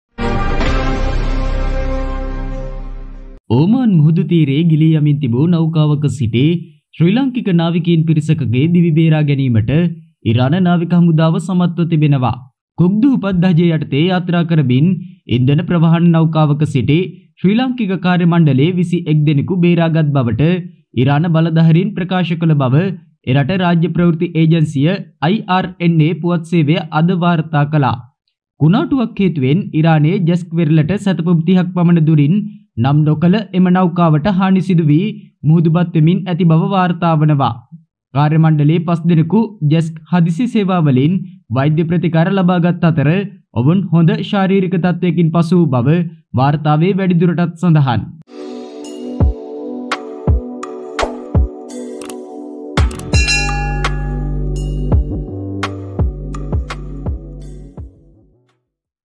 BREAKING | ශ්‍රී ලාංකිකයින් පිරිසකට ඕමාන් මුහුදේදී ඉරානය නිසා අත්වුණු ඉරණම | Sri lankan
Iranian authorities said they rescued 21 Sri Lankan crewmen from a Cook Islands-flagged tanker sinking in the Gulf of Oman, the state news agency IRNA reported today, according to Reuters.

The unnamed tanker suffered damage about 30 miles (50 kilometres) off the coast of Jask in Iran due to a storm, the report said.

Five of the crewmen received medical care from Jask emergency services and were in good physical condition, the report added. It did not say when the rescue took place.